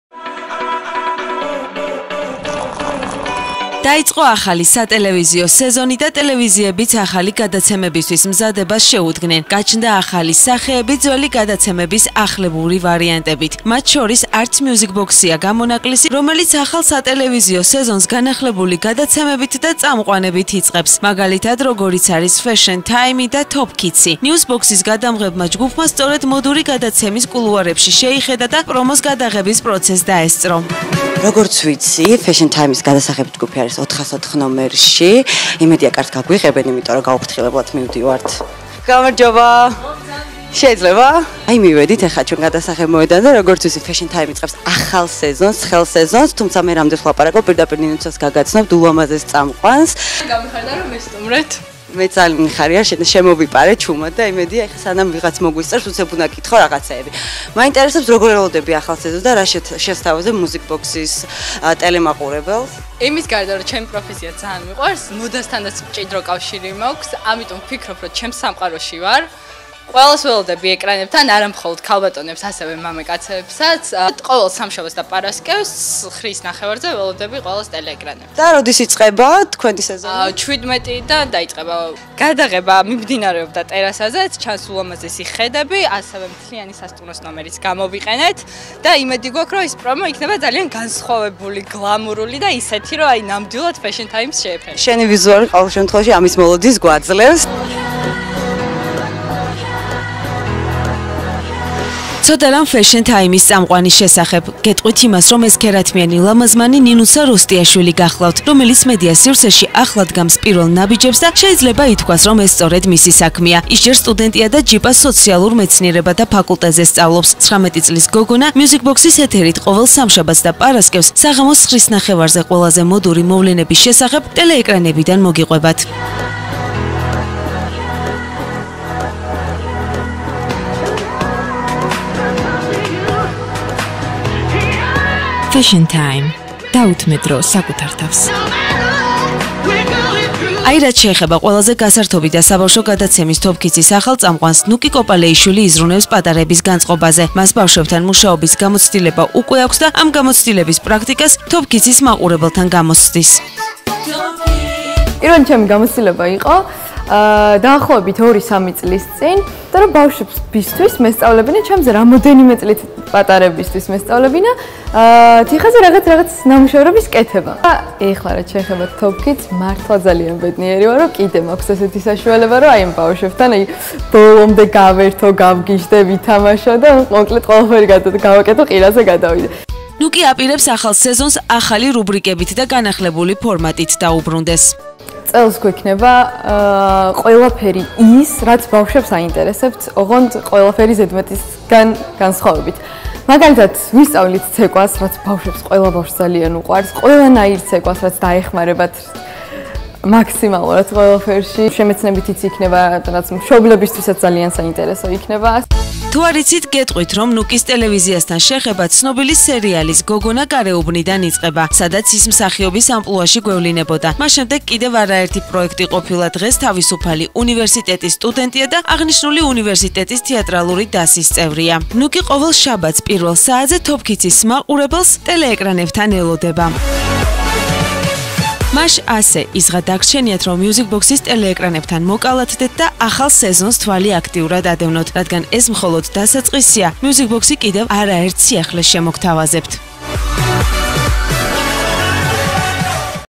ახალი სატელევიზიო სეზონი "Music Box" - ის ეთერში - განახლებული გადაცემები და ახალი წამყვანები
დაიწყო ახალი სატელევიზიო სეზონი და ტელევიზიებიც ახალი გადაცემებისთვის მზადებას შეუდგნენ. გაჩნდა ახალი სახეები, ძველი გადაცემების ახლებური ვარიანტები. მათ შორის არც music box -ია გამონაკლისი